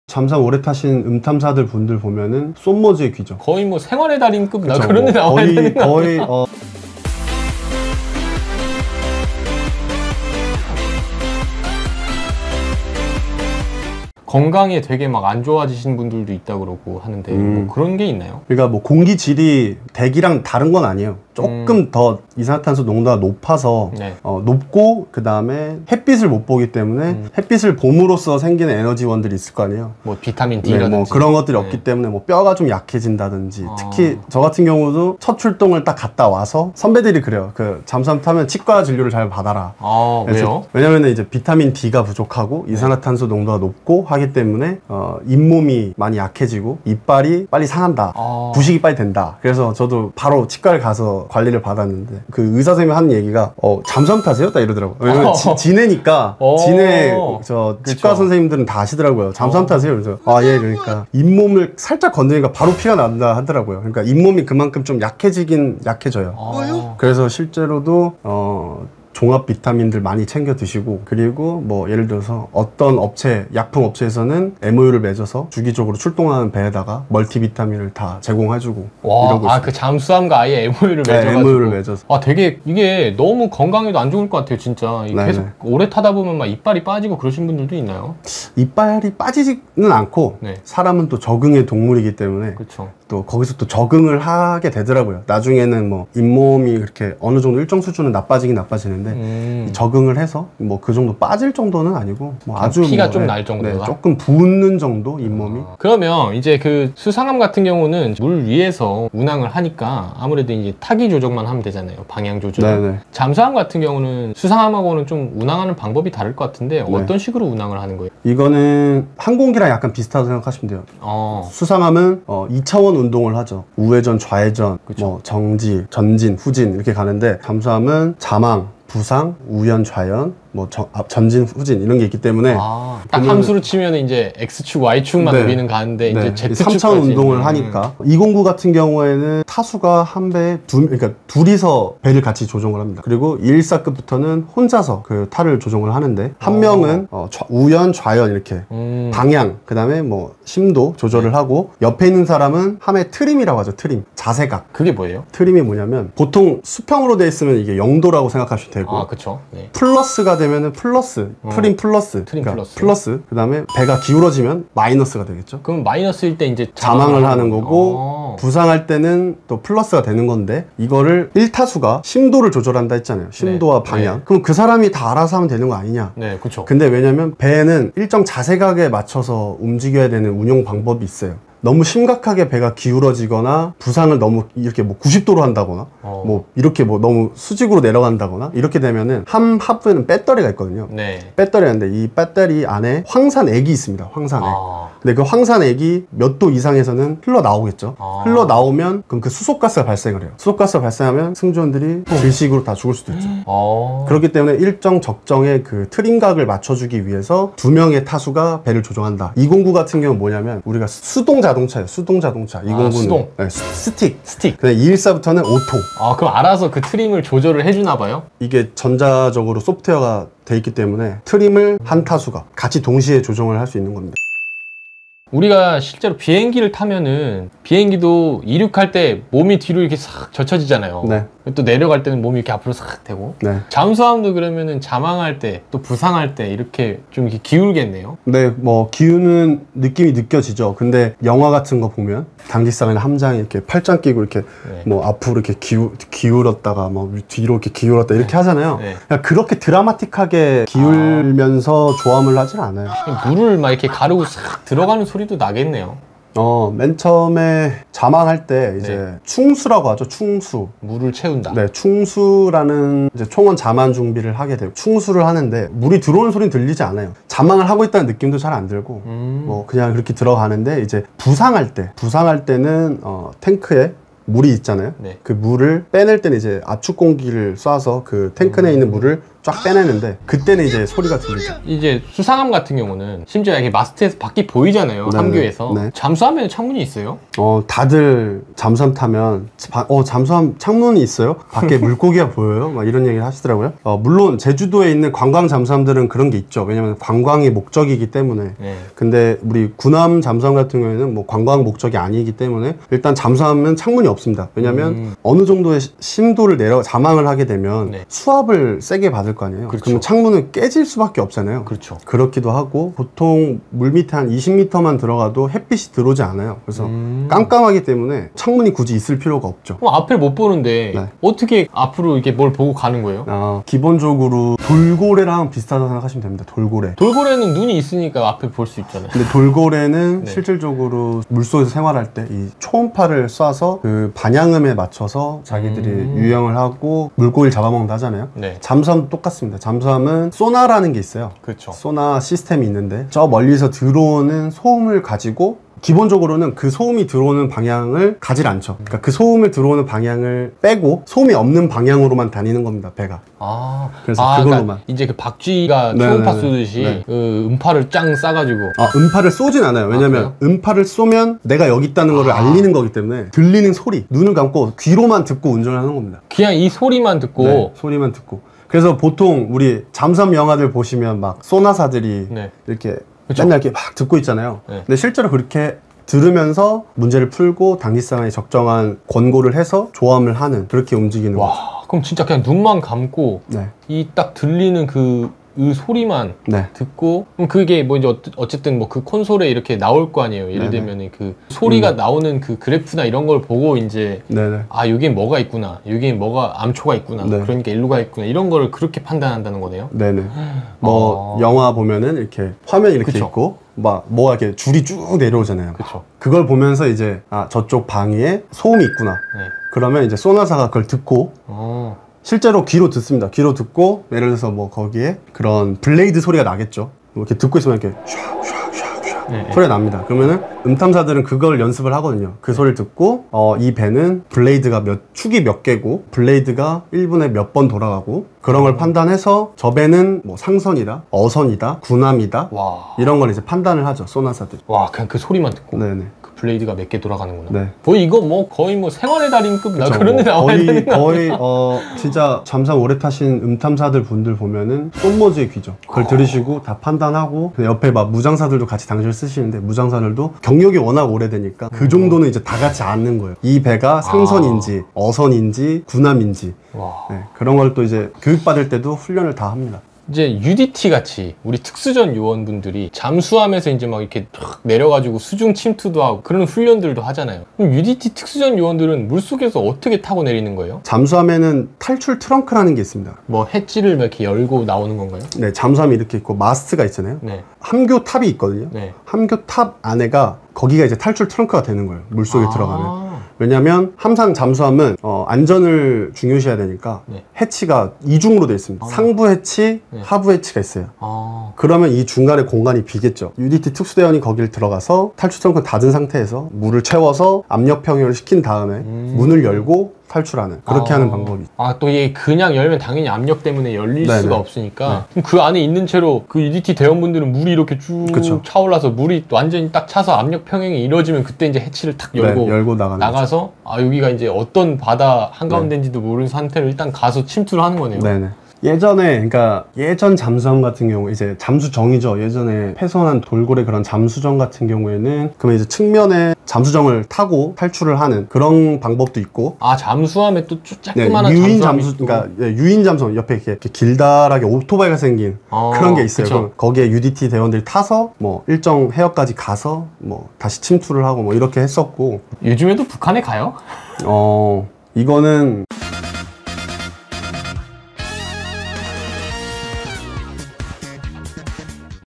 심해잠수함의 놀라운 기동방법[잠수함장교2부]
잠수함에도 창문이 있을까요? 잠수함의 놀라운 기동방법에 대해 알아봅시다

Q. 잠수함 오래타면 건강에 안좋을것같은데..?
Q. 잠수함은 어떻게 운항하는지?
Q. 물속에 들어갈 때 몸이 기울기도 하나요?
Q. 잠수함에도 창문이 있나요?
Q. 특수전요원들은 잠수함을 어떻게 타고내리나요?